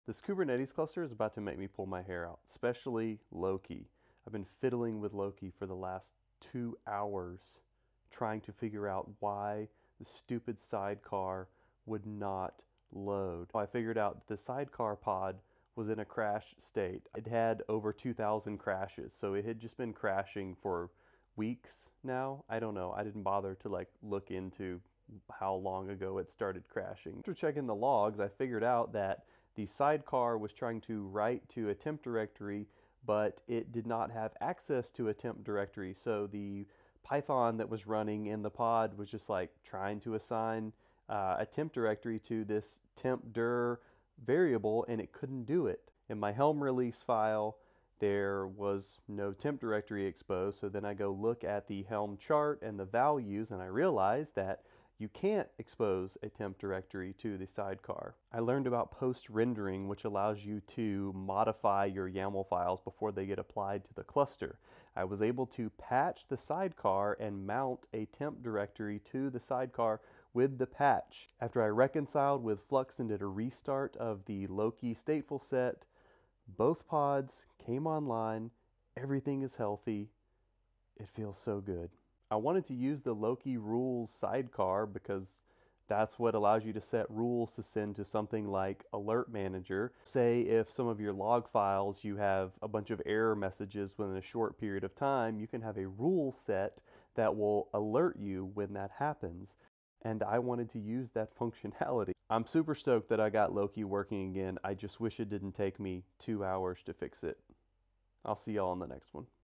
Loki lowkey about to kill me.
Troubleshooting why the Loki sidecar was failing to run in my kubernetes cluster ￼